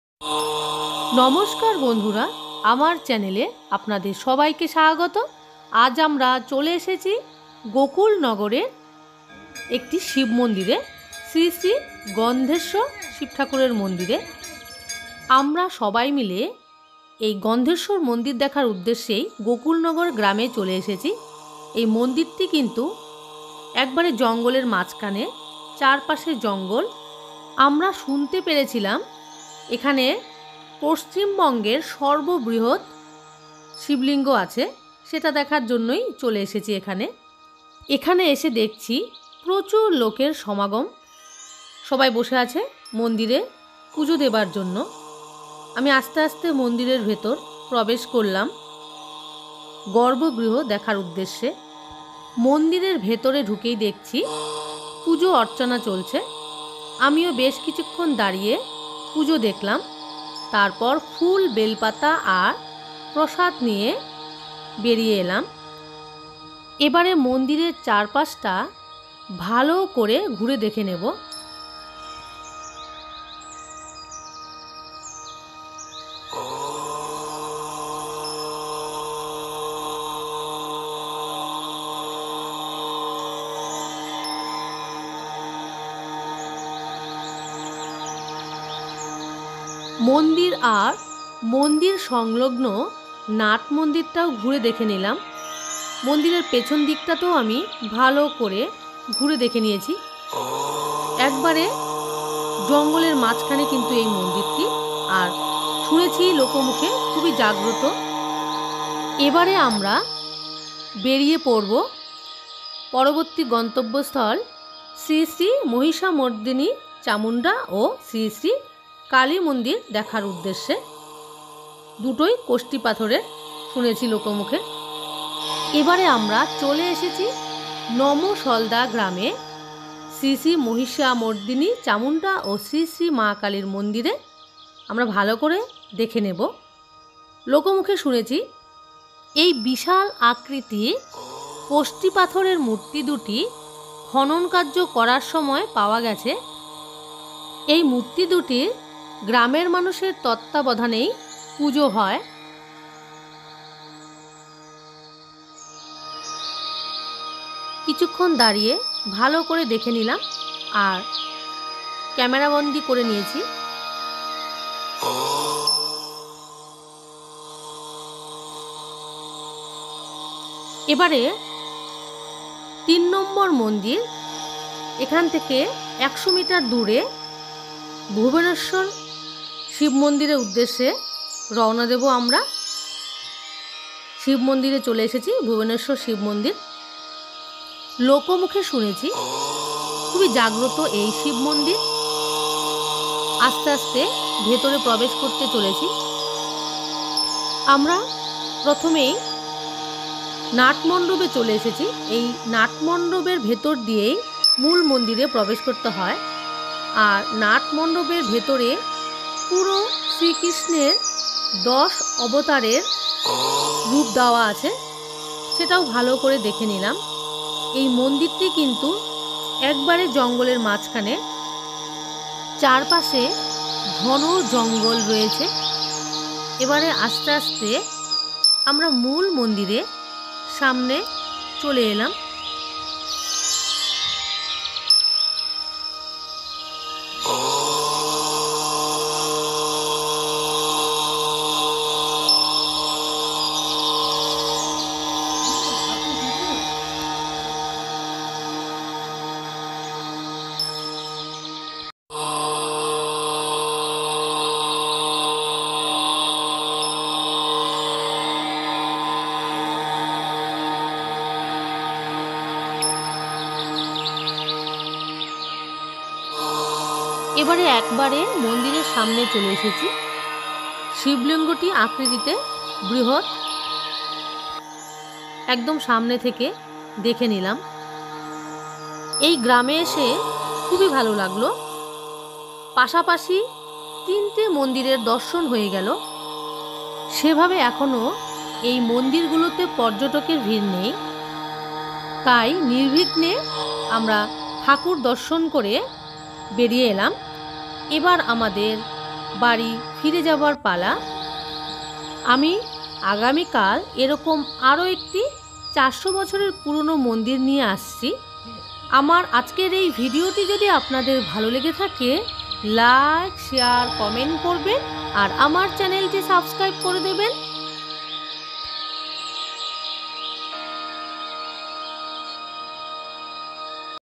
পশ্চিমবঙ্গের সর্ববৃহৎ শিবলিঙ্গ/ gokulnagar/joypur/bankura /travel /vdo /bengalivlogs/@paakshala10
ট্রেন বা বাসে বাঁকুড়ার বিষ্ণুপুর থেকে জয়পুর যেতে হবে। জয়পুর চৌমাথা থেকে গোকুলনগর স্টেশন। সেখান থেকে বা দিকের রাস্তা ধরেই সোজা যেতে হবে।রেলগেট পেরোতে হবে না।